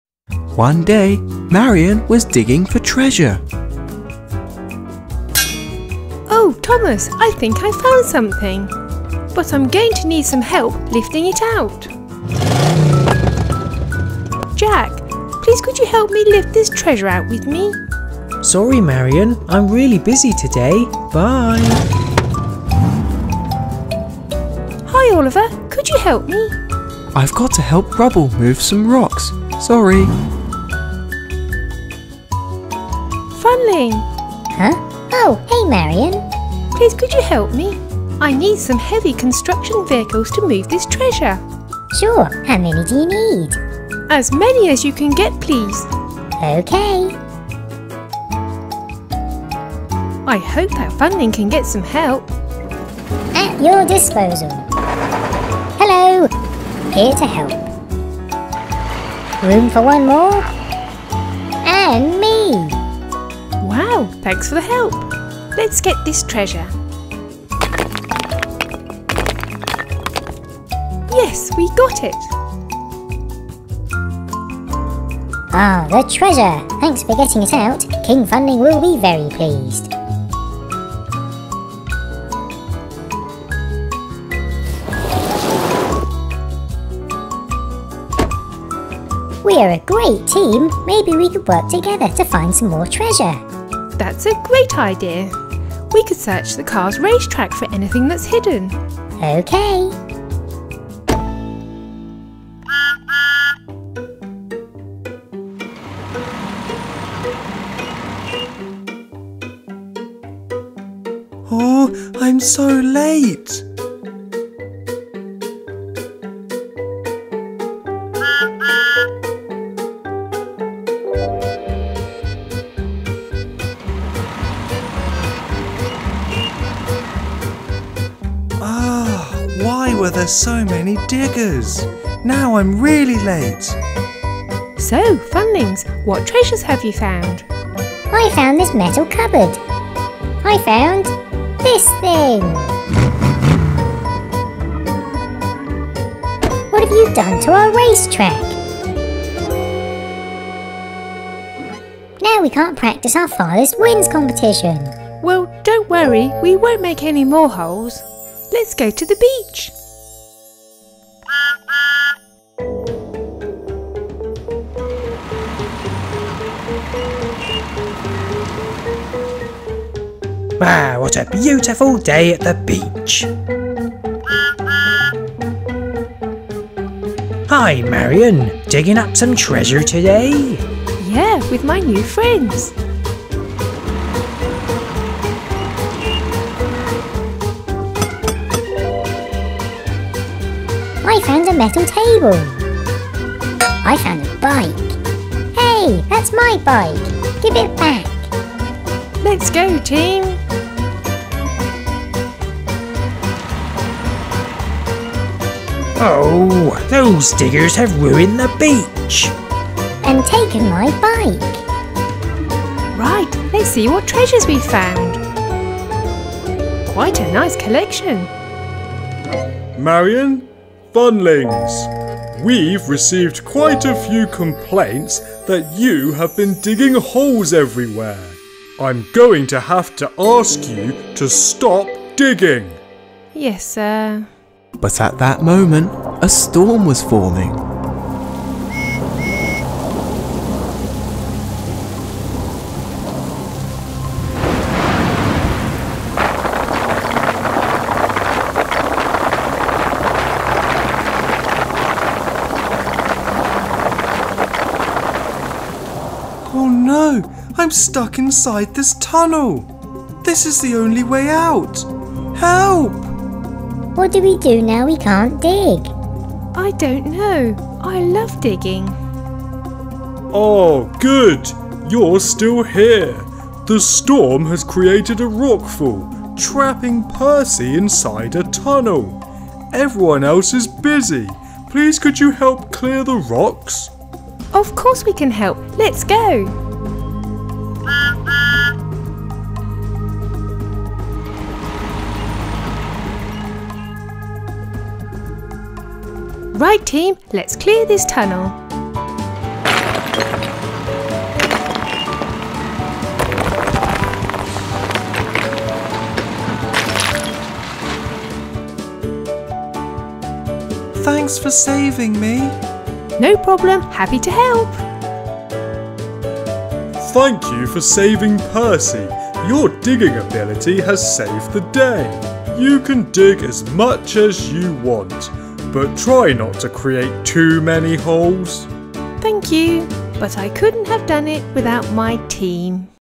Rockfall Story with Marion and Construction Vehicle Toys
Rockfall with Marion and CAT construction vehicle toys. Digs and Discoveries Jack helps Marion dig for treasure but then she is required to carry out a rescue after a rockfall. She get's assistance from the CAT excavators and diggers